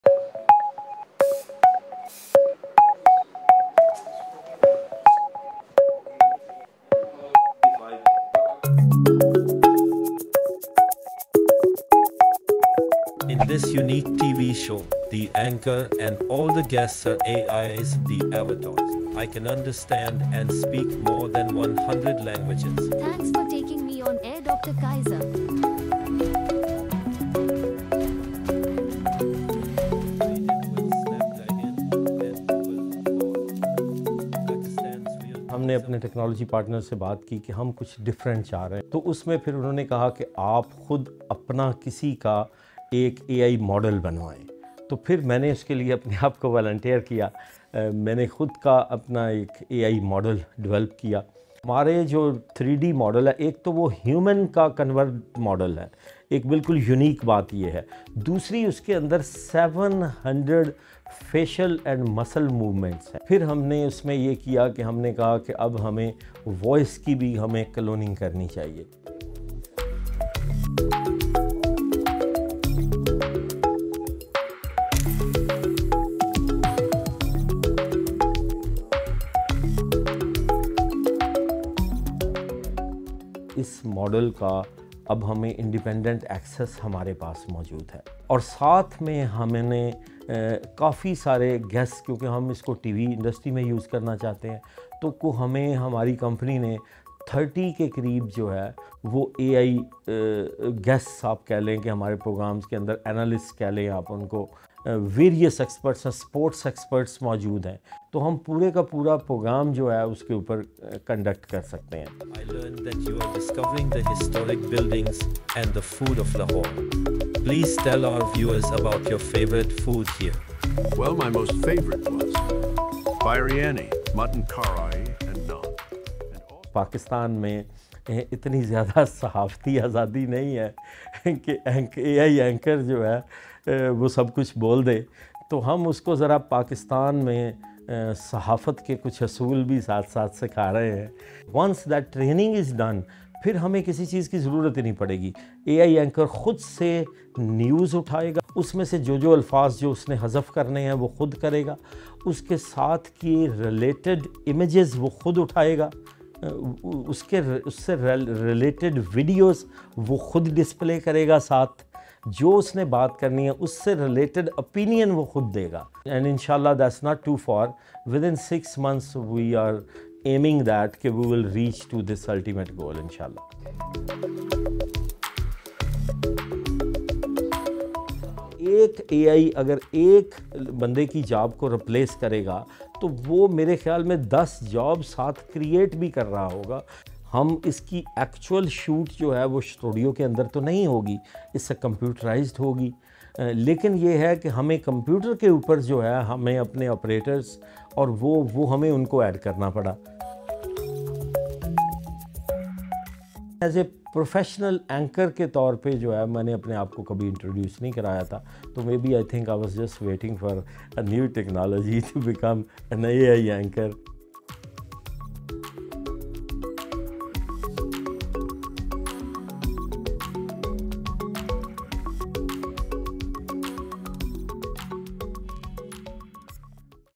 Pakistan's 'First AI Talk Show: Will AI anchors replace humans in the future? - BBC URDU
پاکستان میں ایک انفوٹینمنٹ چینل ’ڈسکور پاکستان‘ نے حال ہی میں ٹی وی پر ایک ٹاک شو کیا جس کے بارے میں ان کا دعویٰ ہے کہ اس میں اینکر اور مہمان تمام اے آئی ماڈلز ہیں یعنی حقیقی انسان کی کمپیوٹر سے بنی شکل اور آواز ہیں۔ یہ ماڈل کس طرح کام کرتا ہے اور کیا اے آئی اینکرز مستقبل میں جیتے جاگتے اینکرز کی جگہ لے لیں گے؟ جانیے عمردراز ننگیانہ اور فرقان الہٰی کی اس ویڈیو میں